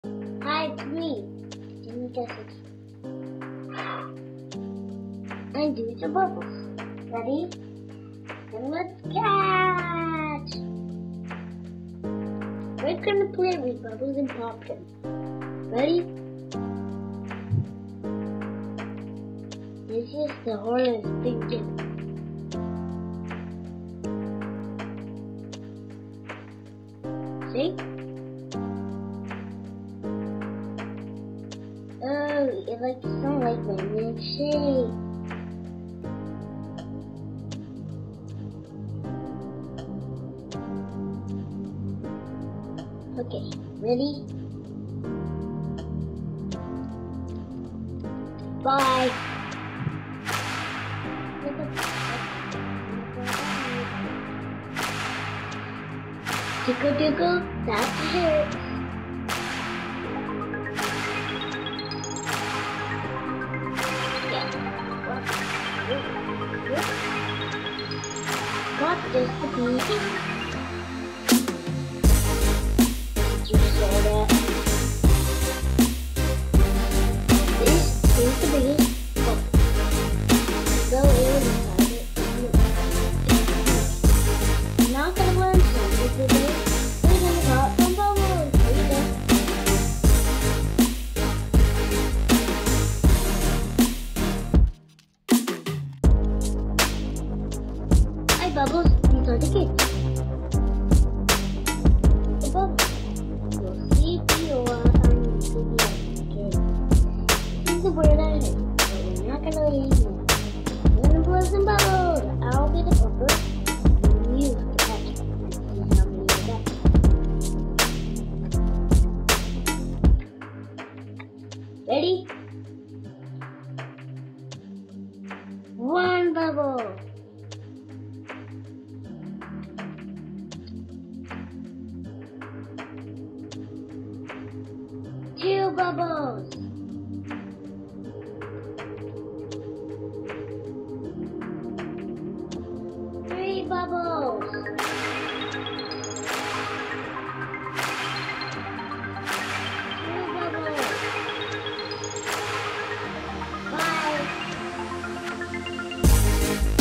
How Bubbles Work: Watch 5-year-Old Bubble Talk Show Episode | Counting Numbers with bubbles
5yr old shares his favorite thing to do with his audience figuring out how bubbles work, counting, and popping